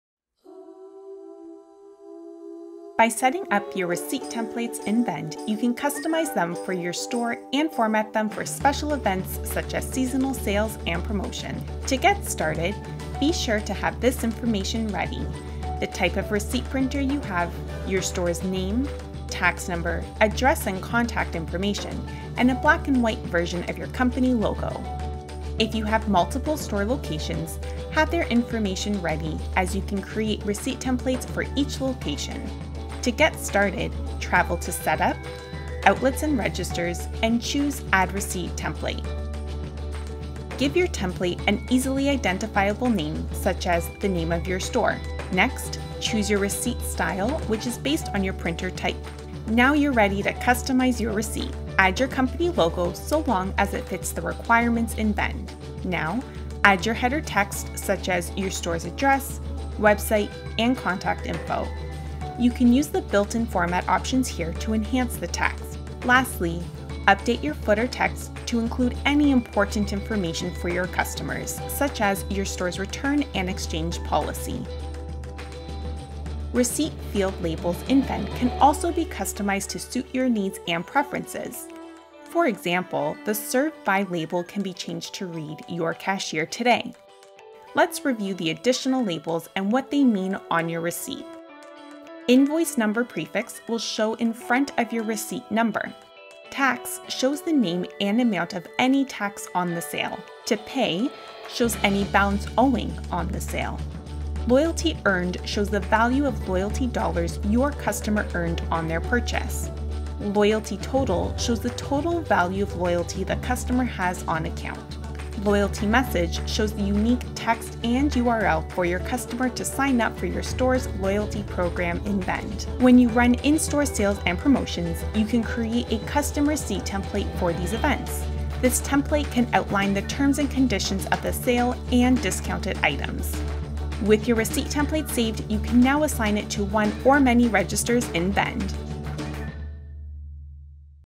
Receipt Customization in Vend
Receipts can be customized to your store's and customers needs, and can be associated with different Outlets and Registers.